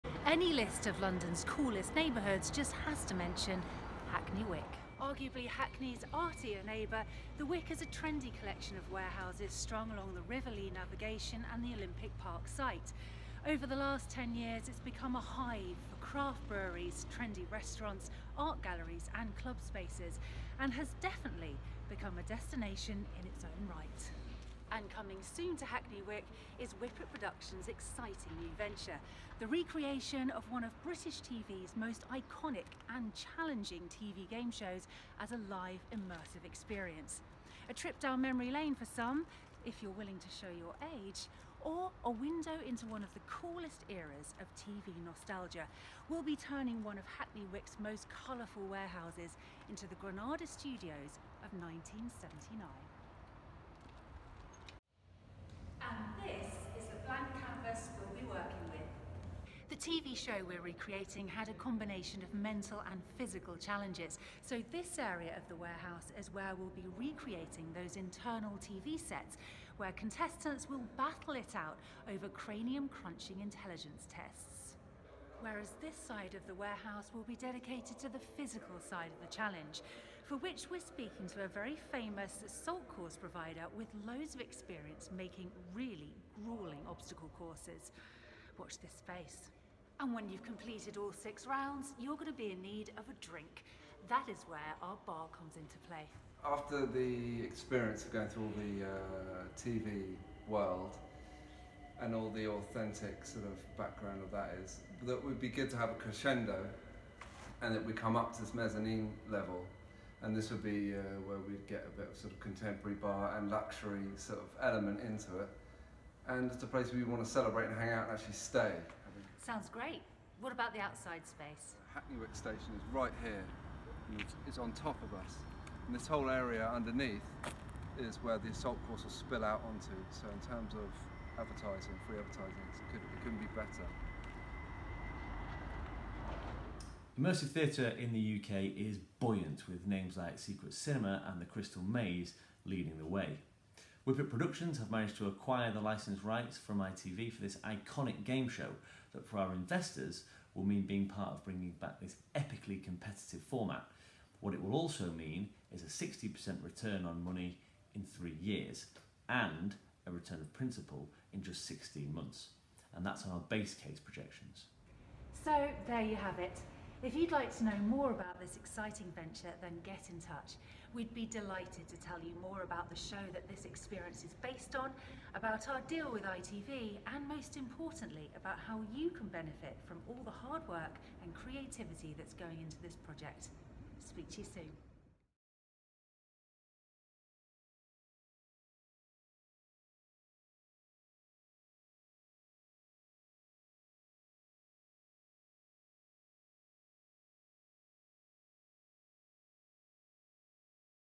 Freya Berry & Rick Behan introduce a new secret immersive event
Interested in alternative investments? Want to find out more about investing in theatre and entertainment? Bringing back one of ITV's most iconic and challenging TV gameshows as an exhilarating live event, Whippet Productions are taking over this former T-shirt printing factory in Hackney Wick to create London's next big immersive show.